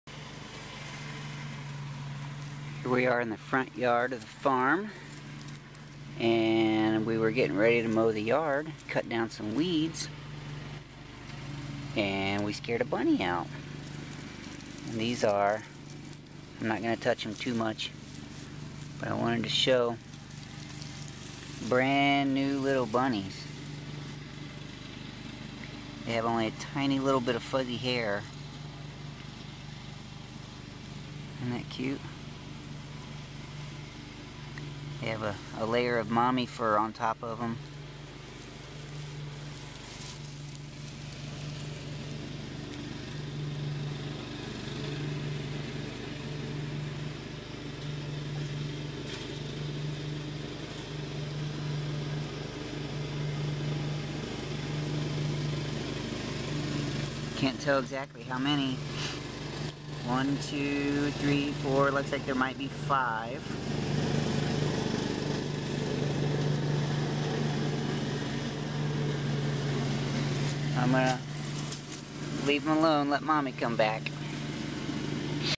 Fresh Baby Bunny Rabbits in Nest
While discussing where to mow we noticed several rabbits hanging out in the tall grass.  I went to check it out and found this!  We steered clear of the area and left the grass tall.